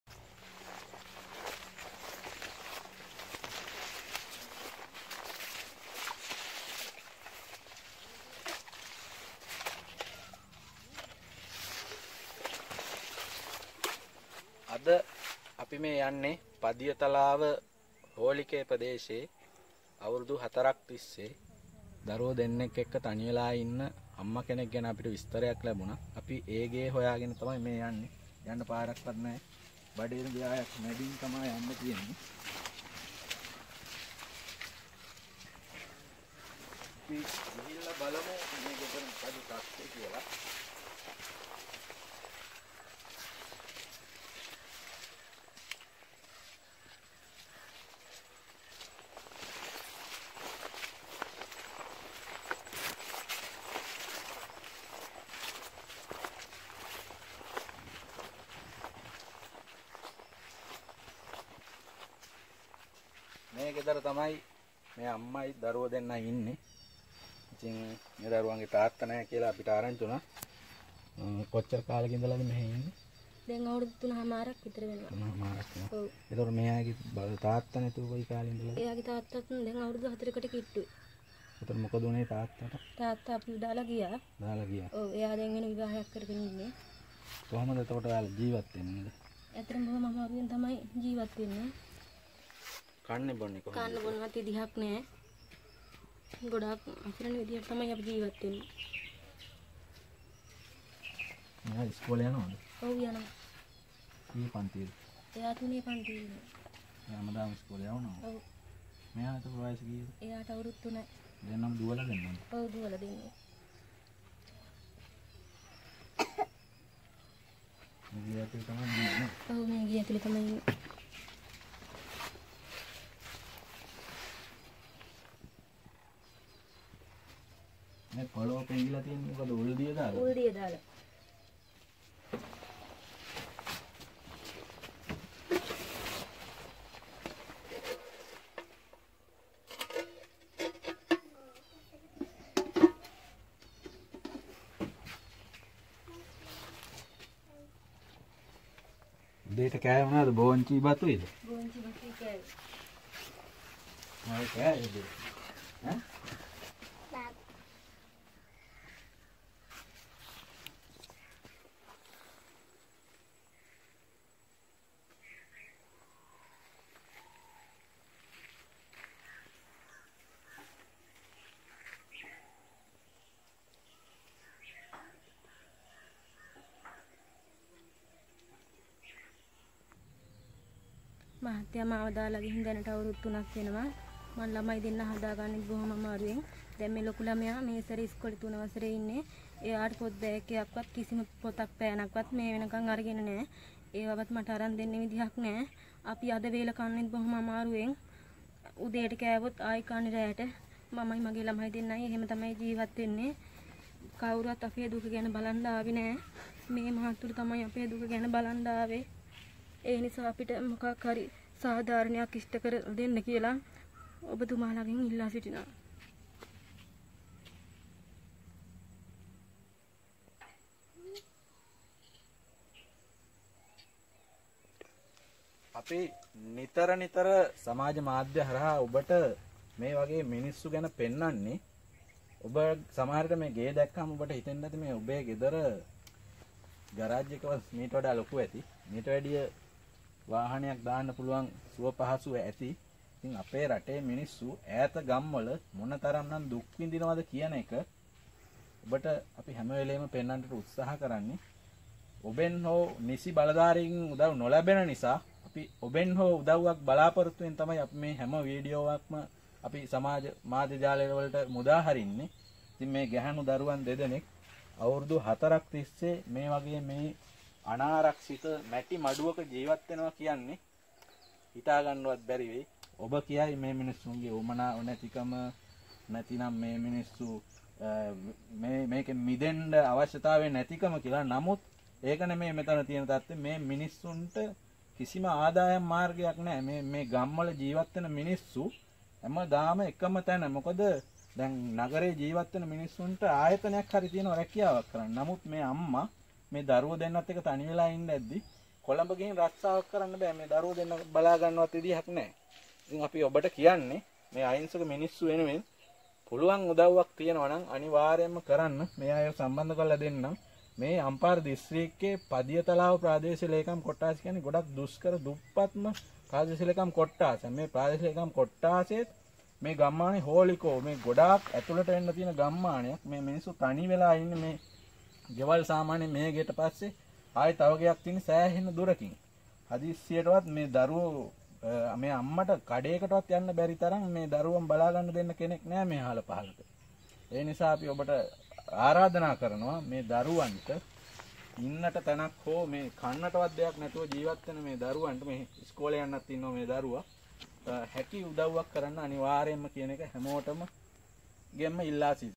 දරුවන් හදා හැර යන මිනිසුන් නිසා දුක්විදින අම්මලා සහ දරුවෝ | Another helpless family
අසරණ මවගේ ගිණුම් අංකය

T M I S Kumari
Boc Bank
Padiyathalawa Branch
85296148